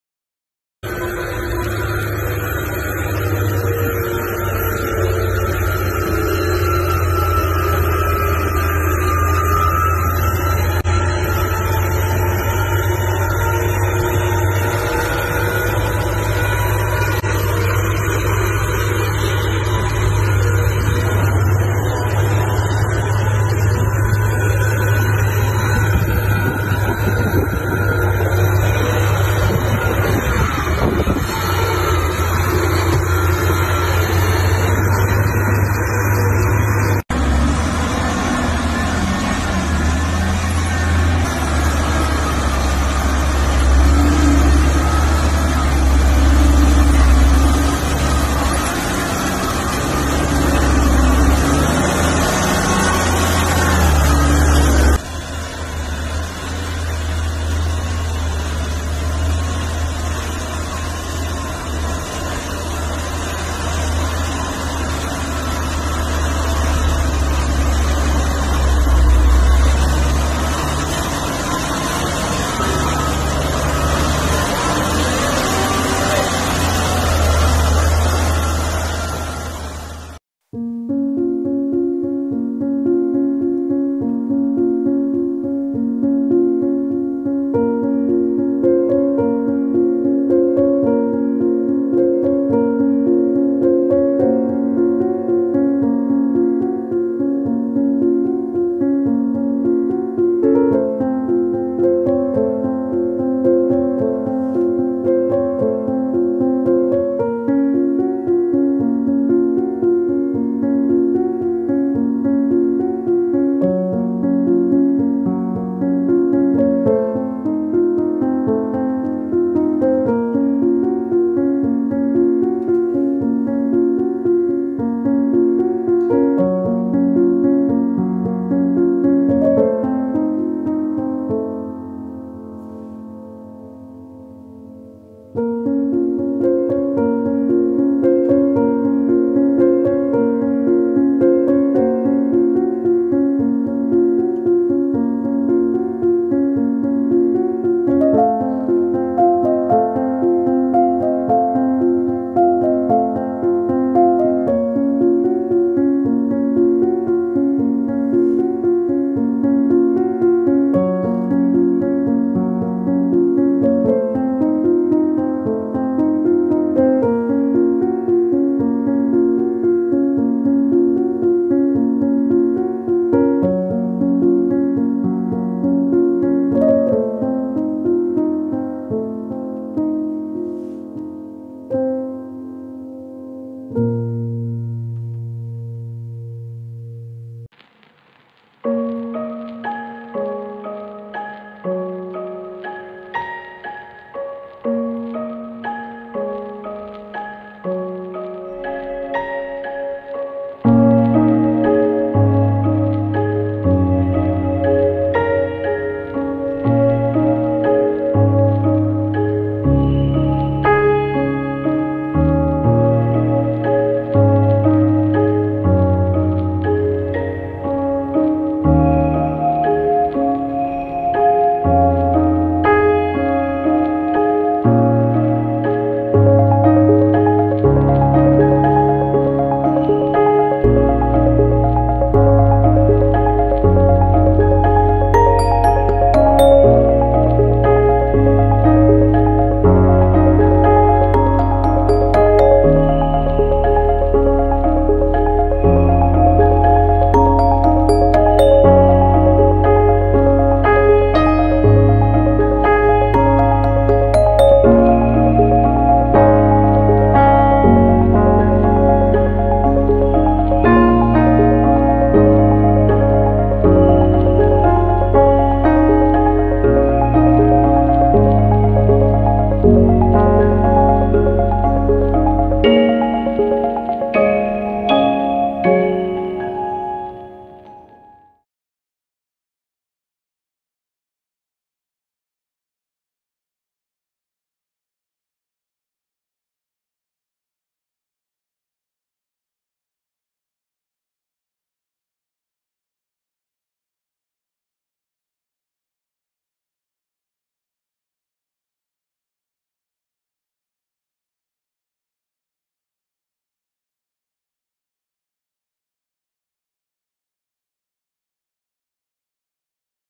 Daring truck driver driving heavy over loaded trucks
All contents provided on this channel is created by ME(KyongMyung)